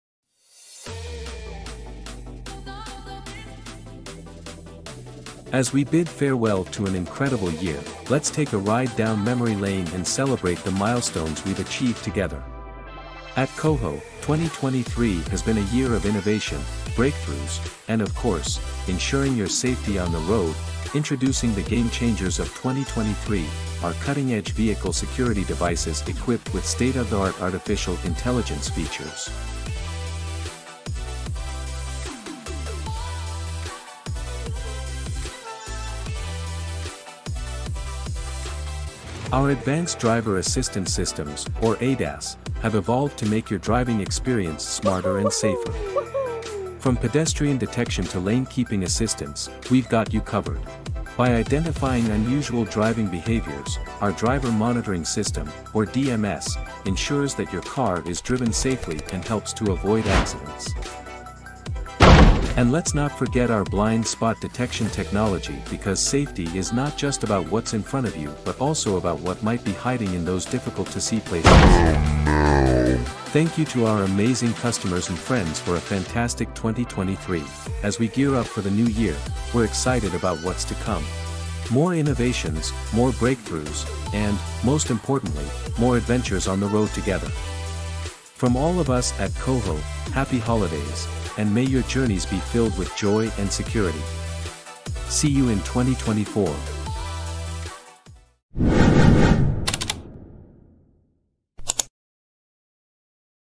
myyearonyoutube2023 Smart Mobile DVR makes your more Safe Driving.
As we bid farewell to an incredible year, let's take a ride down memory lane and celebrate the milestones we've achieved together.
At QOHO, 2023 has been a year of innovation, breakthroughs, and of course, ensuring your safety on the road!

 Introducing the game-changers of 2023 - our cutting-edge vehicle security devices equipped with state-of-the-art Artificial Intelligence features!

Our Advanced Driver Assistance Systems, or ADAS, have evolved to make your driving experience smarter and safer. From Pedestrian detection to lane-keeping assistance, we've got you covered.
By identifying unusual driving behaviors, our Driver Monitoring System, or DMS, ensures that your car is driven safely and helps to avoid accidents.
And let's not forget our blind spot detection technology because safety is not just about what's in front of you but also about what might be hiding in those difficult-to-see places

Thank you to our amazing customers and friends for a fantastic 2023. We couldn't have done it without you
As we gear up for the new year, we're excited about what's to come. More innovations, more breakthroughs, and, most importantly, more adventures on the road together.

From all of us at QOHO happy holidays, and may your journeys be filled with joy and security. See you in 2024!